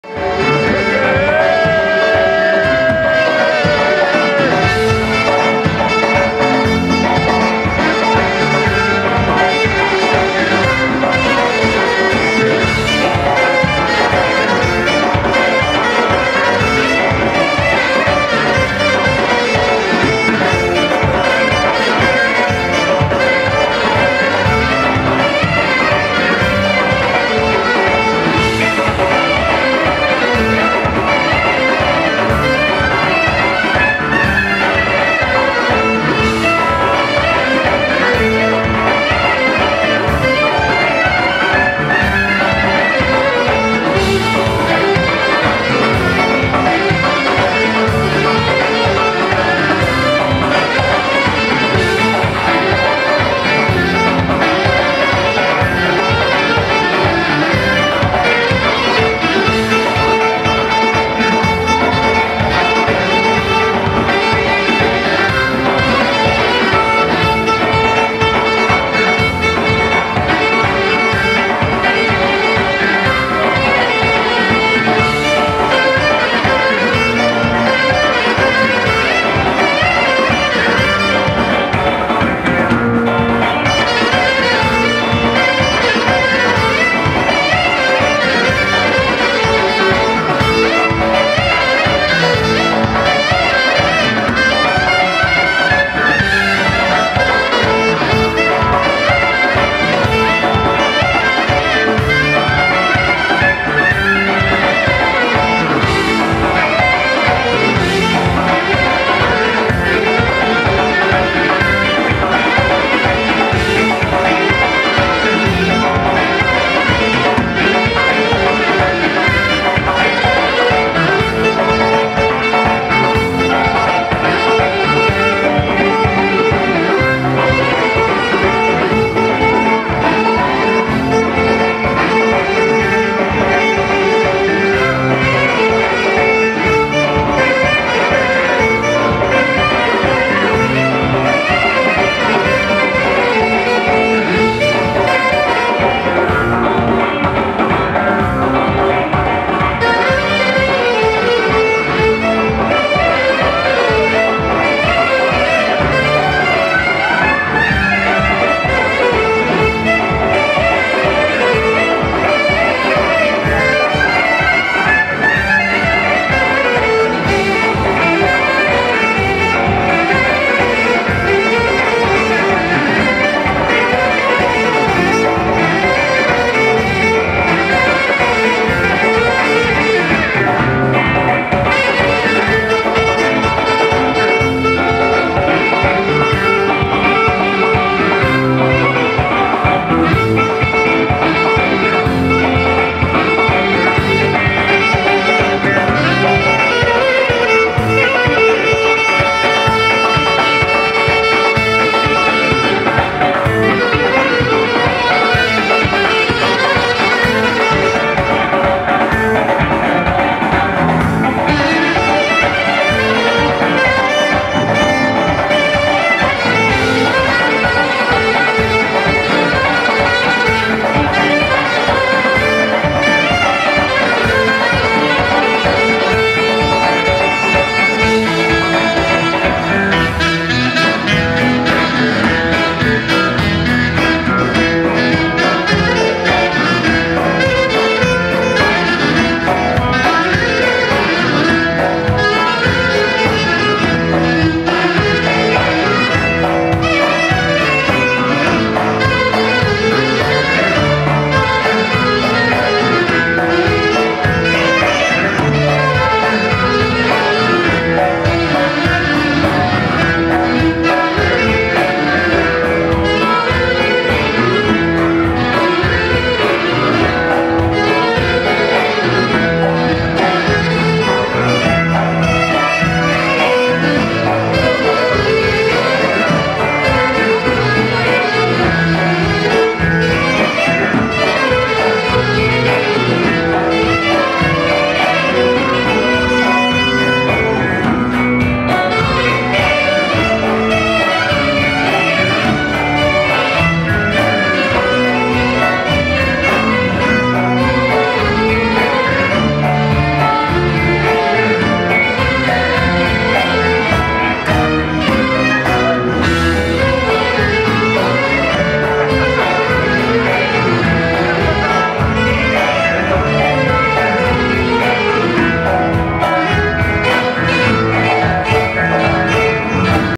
Μάγεψε με το κλαρίνο και χειροκροτήθηκε ο Ταξιάρχης Βλαχοδημος
Μάγεψε με το κλαρίνο  και χειροκροτήθηκε  ο Ταξιάρχης Βλαχοδημος στην κοπή πίτας του   Περιβαλλοντικού συλλόγου Χρυσοβίτσας